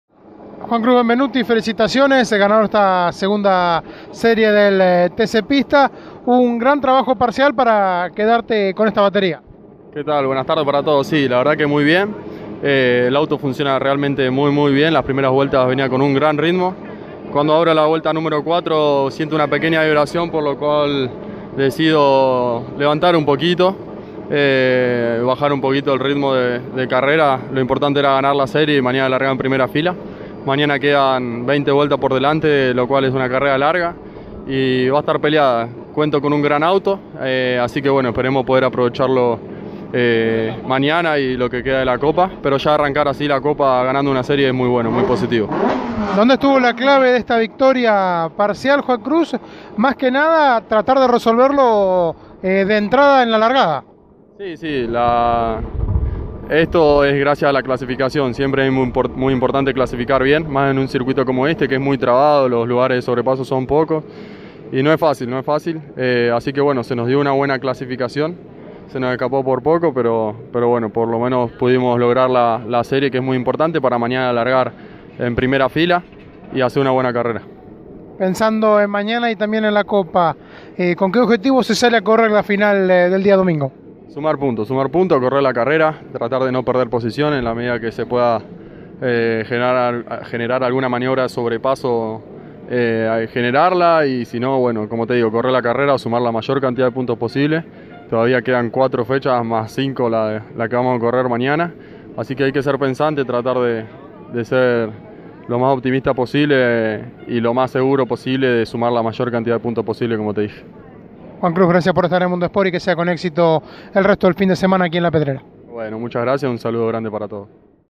Juan Cruz Benvenuti - Serie 2 TC Pista La Pedrera 2018
Luego de quedarse con la segunda serie del TC Pista en el autódromo de La Pedrera, Juan Cruz Benvenuti pasó por los micrófonos de Mundo Sport y comentó sus sensaciones.